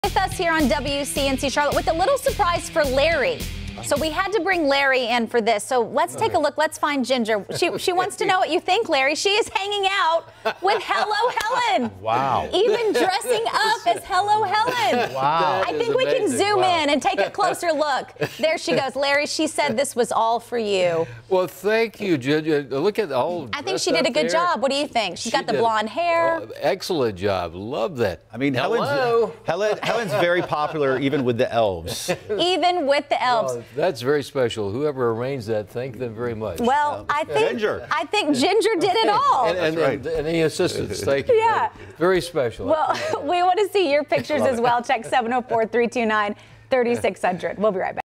Ginger the Elf surprises Larry Sprinkle by dressing up as Hello Helen
Ginger the Elf had a surprise for @sprinkle_larry today, and dressed up as Hello Helen! But the fun didn't stop there -- a very special guest called during WakeUpCLT To Go Wednesday morning.

We want to see your Elf on the Shelf photos!